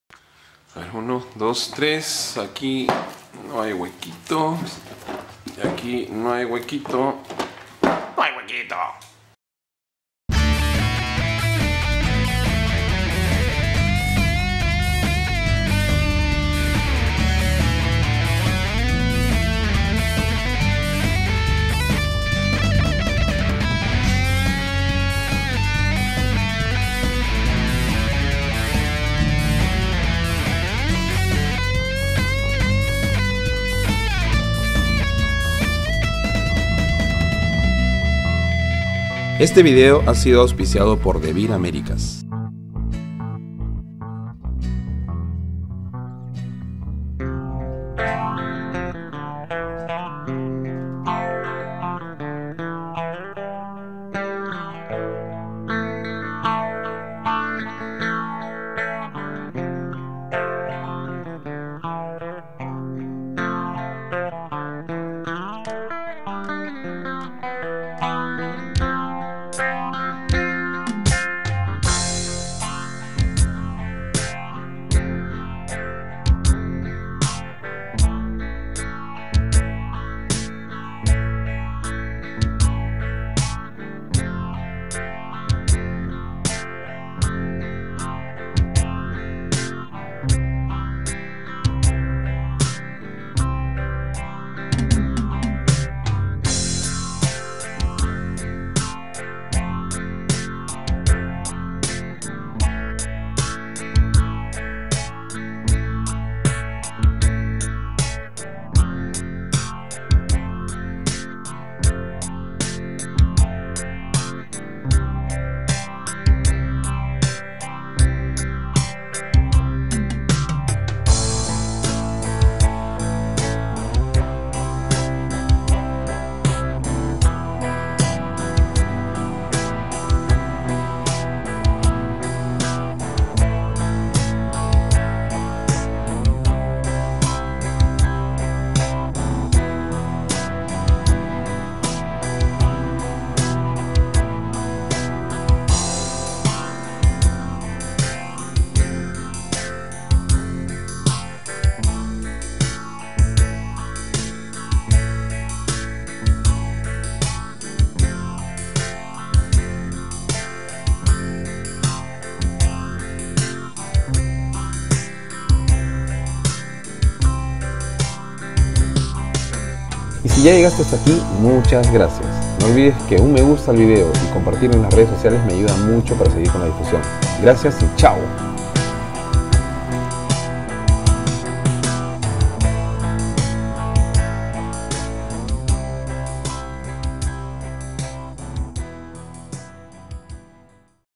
Unboxing: Ubongo
Trepidante, adictivo Ingenioso... Aquí el contenido de este juego con el que te enganchas a la primera!